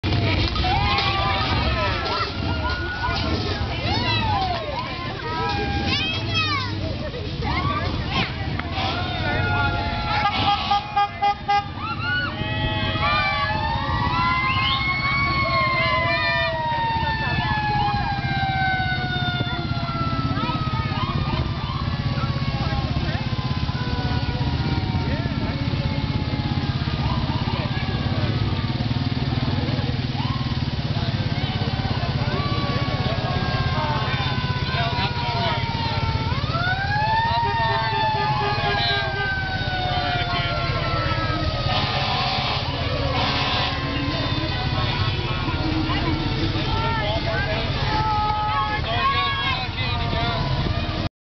Rockwall 4th of July Parade - Party Boat and Rockwall Rangers
Baseball Champs Rockwall Rangers & Pontoon Party Boat